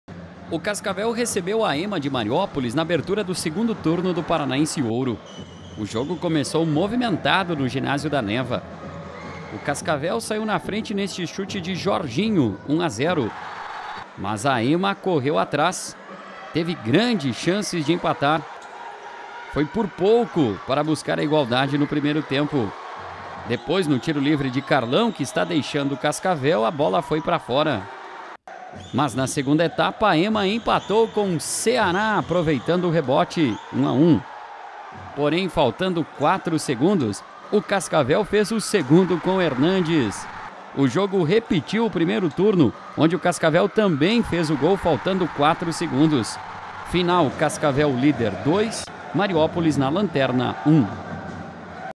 Cascavel vence sobre Mariópolis
Esporte: Em jogo que repetiu o que foi registrado no primeiro turno, o Cascavel venceu o Mariópolis com gol a quatro segundos do final da partida.